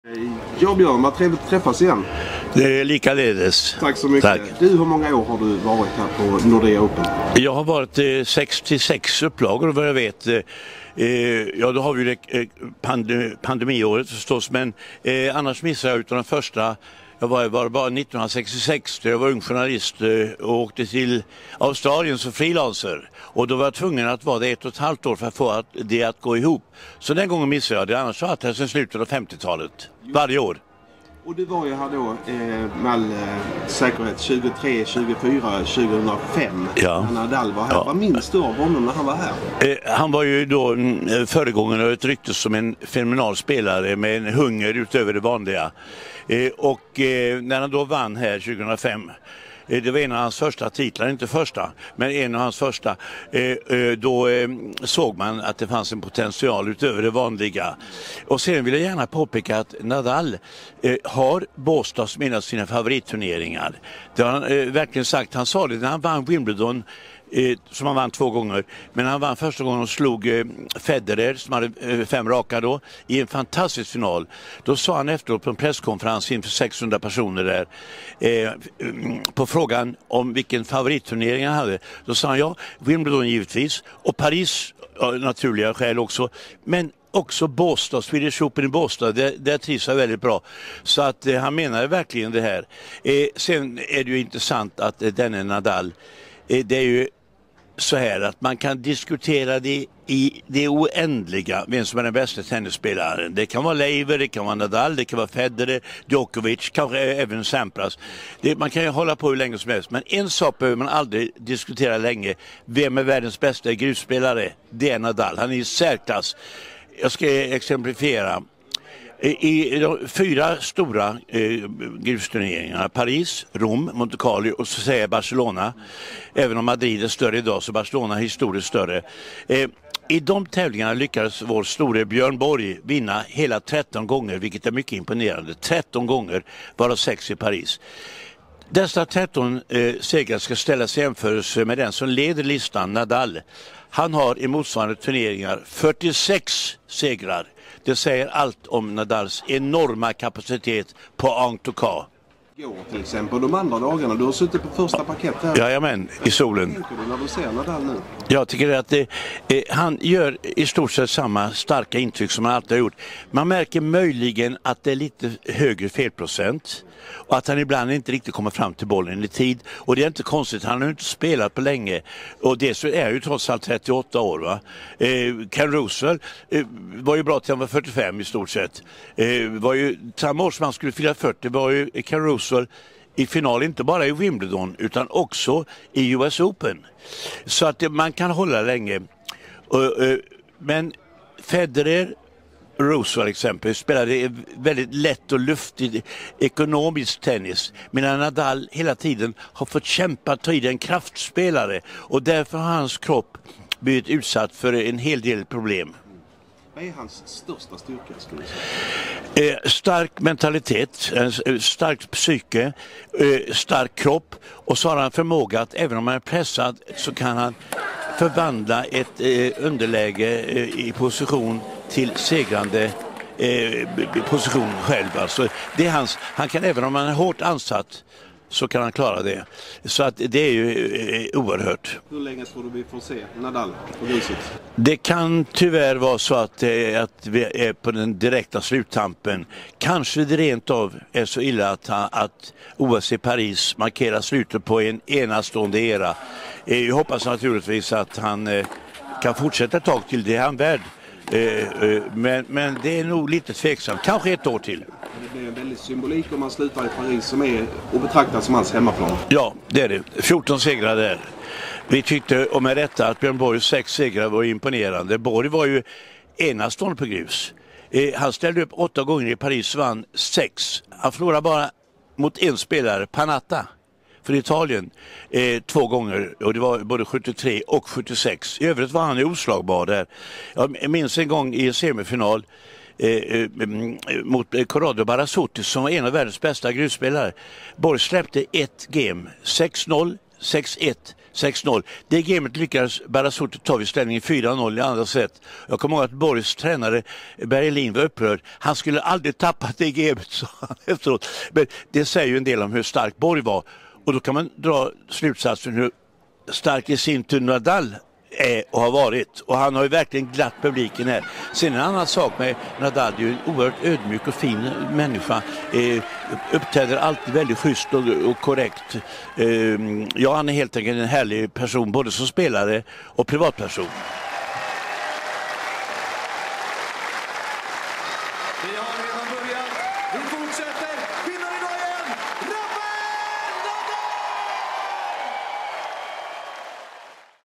Hallå där, Björn Hellberg!
Legendariske tennisjournalisten Björn Hellberg har varit på plats under tennisveckorna i Båstad varje år sedan slutet av 1950-talet (förutom 1966 och 2021 (pandemin). Det har i alla fall blivit 66 upplagor på plats i Båstad - troligen mer än någon annan. Vad minns han från åren när Rafael Nadal var i Båstad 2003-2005 och vilka slutsatser drar han när han nu såg honom 2024?